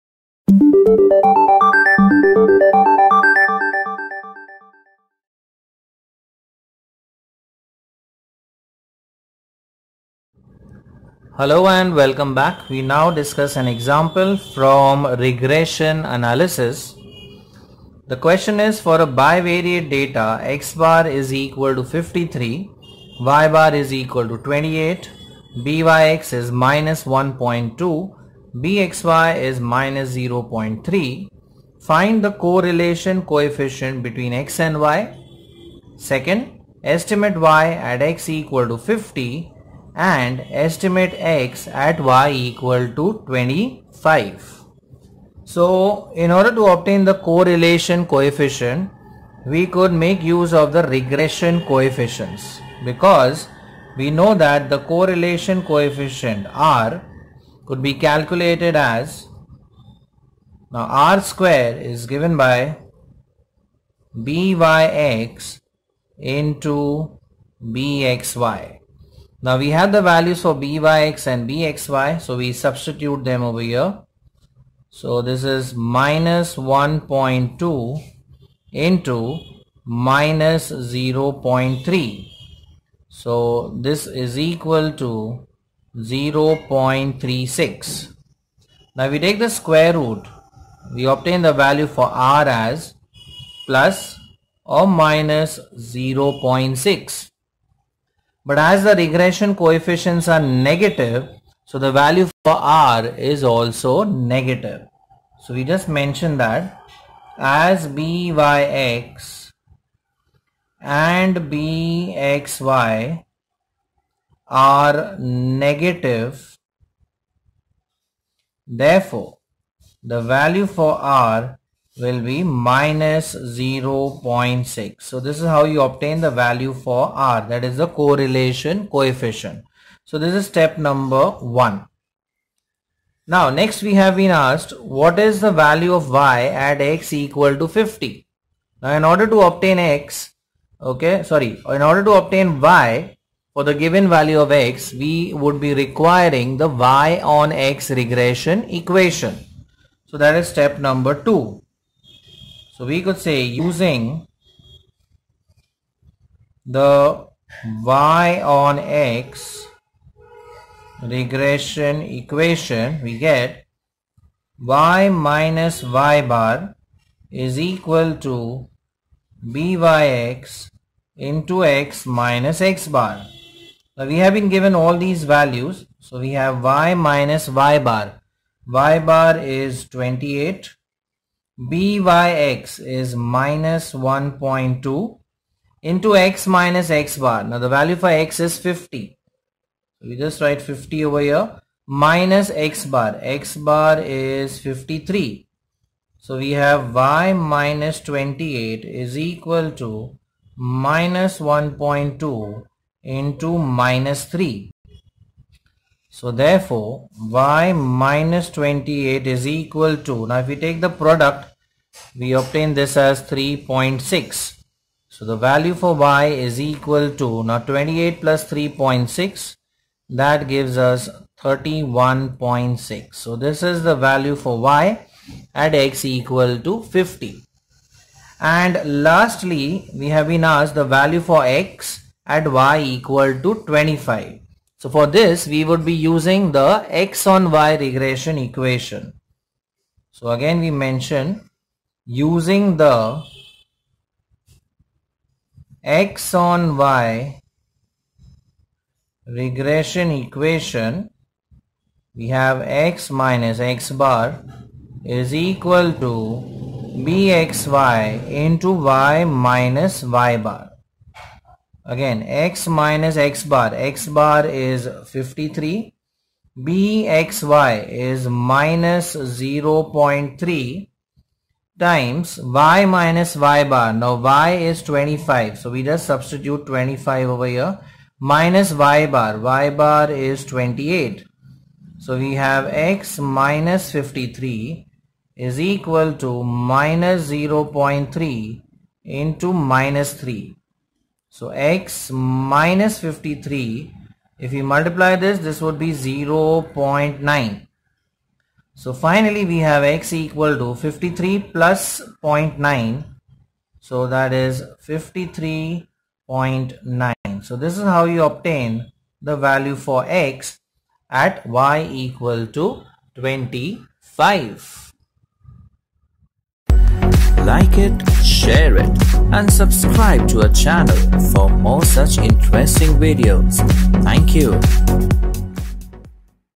Regression Analysis - Linear Regression - NCERT, ISC, ICSE, CBSE, CA, GCSE, IGCSE, IB - Example #13
For a bivariate data:
X̅= 53, Y̅= 28, byx = −1.2, bxy = −0.3
Find  i)  Correlation coefficient between X
and Y.
         ii)  Estimate of Y for X = 50
        iii)  Estimate of X for Y = 25